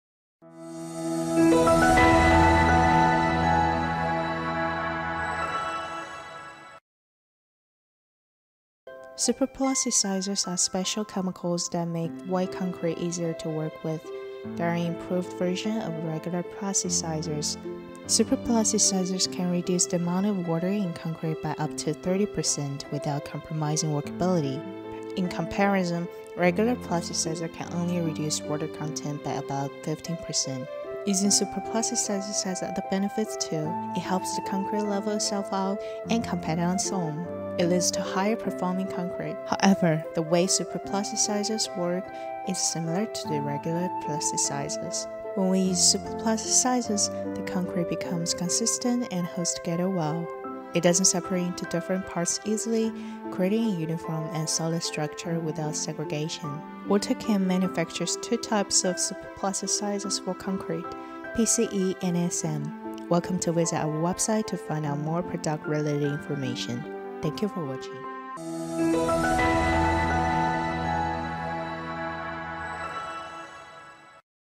Plasticizers vs. Superplasticizers: Concrete Enhancement Secrets
Unleash the full potential of your concrete mixes with superplasticisers. This video delves into the science behind superplasticizers and their role in enhancing concrete workability and strength. Discover the difference between plasticizer and superplasticizer. And see how these additives enable you to create high-performance concrete structures with reduced water content, improved flow, and exceptional durability.

Timecodes:
0:00 - 0:17 Intro
0:17 - 0:45 Differences between plasticizer and superplasticizer
0:45 - 0:56 Superplasticizer for concrete
0:56 - 1:15 Contact WOTAIchem